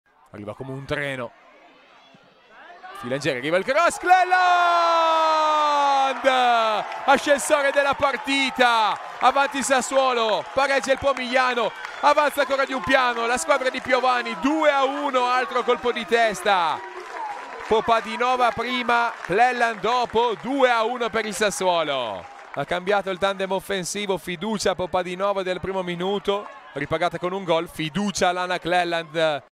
Il gol decisivo di Clelland in SassuoloPomigliano
Oltre ad aver regalato i primi tre punti alle neroverdi, l'attaccante scozzese ha effettuato più tiri di tutte nello specchio della porta.